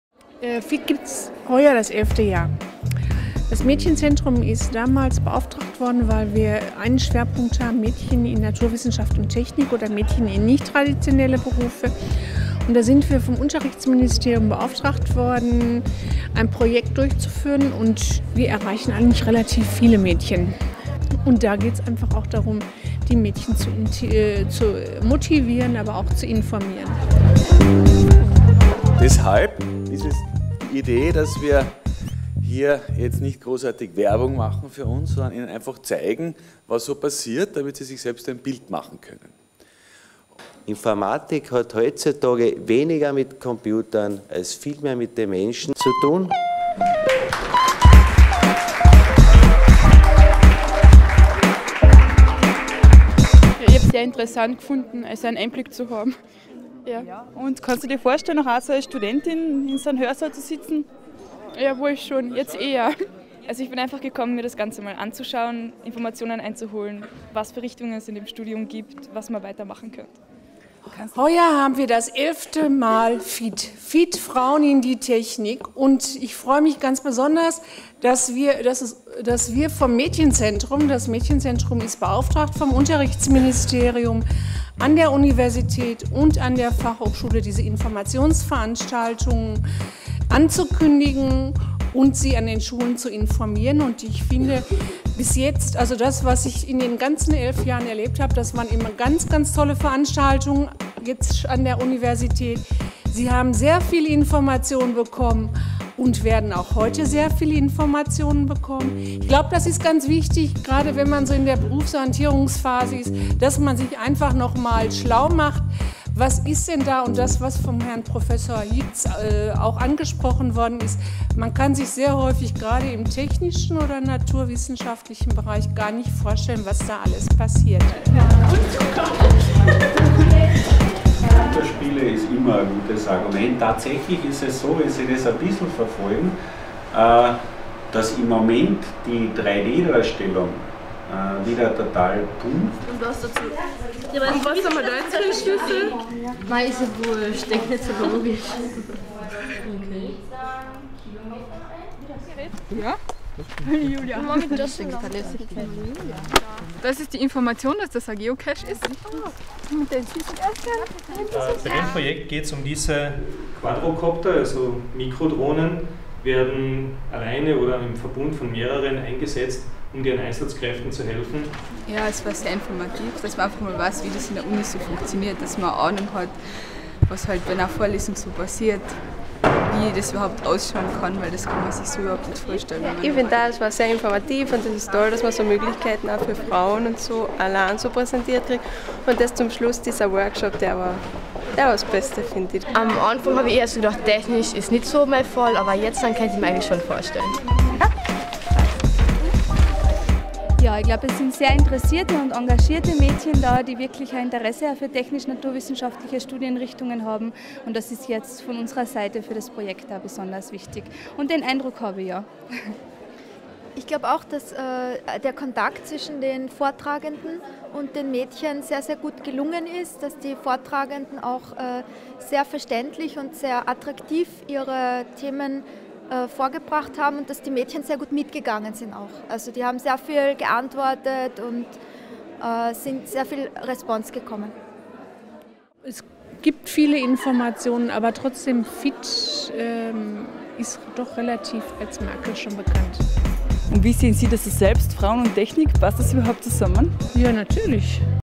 FIT Day University Klagenfurt
Taster day for girls at the University of Klagenfurt - Lakeside Labs support the initiative
Women in technical subjects are still the exception. Although in the last years we can see a rising tendency. The initiative "FIT - Women into Technology" took an important part of this development.
This year the orientation days took place again in the first week of February. The University of Klagenfurt hosted the event on February 1st.
This project aimed to raise awareness among schoolgirls and young women of the opportunities for studying technical subjects at an University.